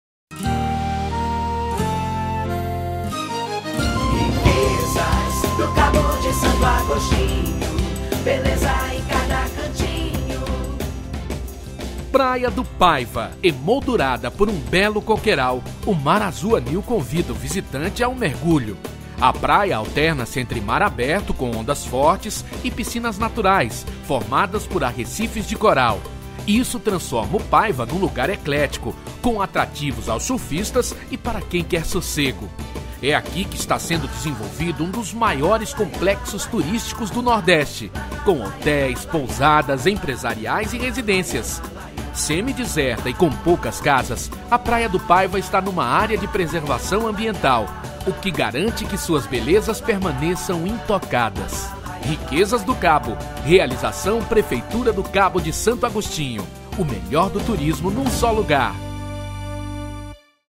Praia do Paiva • Riquezas do Cabo de Santo Agostinho
Praia do Paiva

Emoldurada por um belo coqueiral ao fundo e um mar bem azul à frente, a Praia do Paiva alterna-se entre mar aberto com ondas fortes e piscinas naturais formadas pelos arrecifes, ideal para surfistas e banhistas. A sua paisagem é composta, ao sul, pelas praias de Itapuama e Pedra do Xaréu. Sossegada, semideserta e com poucas casas.

Framed by clear blue waters and a lovely grove of coconut trees, the beach at Paiva features both strong waves and calm natural pools formed by coral reefs, a perfect spot for surfers and bathers. Visitors can enjoy views of the beaches of Itapuama and Ponta do Xaréu to the south. Peaceful and semi-deserted.